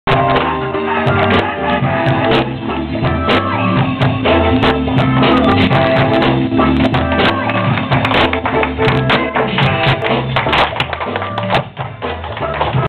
hip hop avec les filles de sainte savine
l'ems a reçut un prof de hip hop sourd et franchement ce mec il vaut le deplacement   CHAPEAU ADAMO